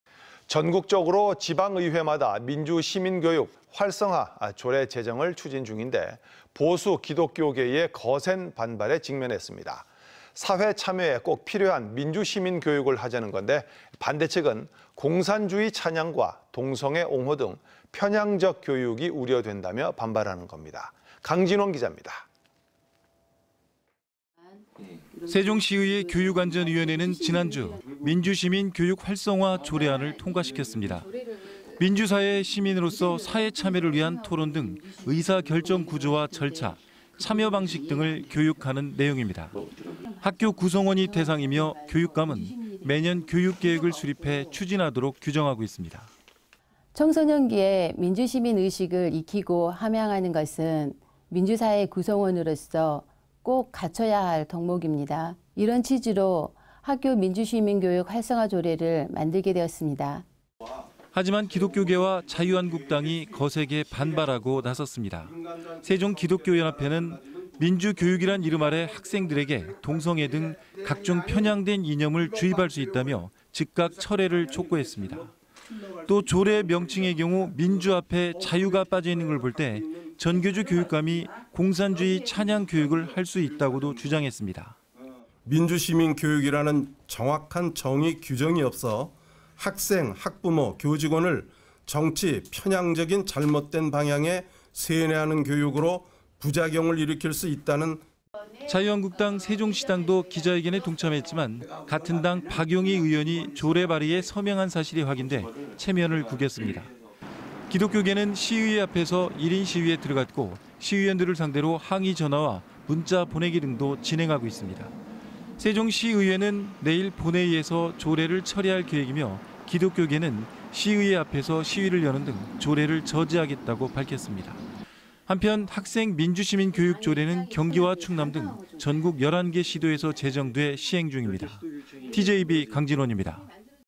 [TJB뉴스] 민주시민교육 조례, 보수 기독교계 '충돌'
전국적으로
지방의회마다
민주시민교육
활성화 조례 제정을
추진중인데,
보수 기독교계의
거센 반발에 직면했습니다

사회참여에 꼭 필요한
민주시민 교육을 하자는건데
반대측은 공산주의 찬양과
동성애 옹호 등
편향적 교육이 우려된다며
반발하는 겁니다.

강진원 기자.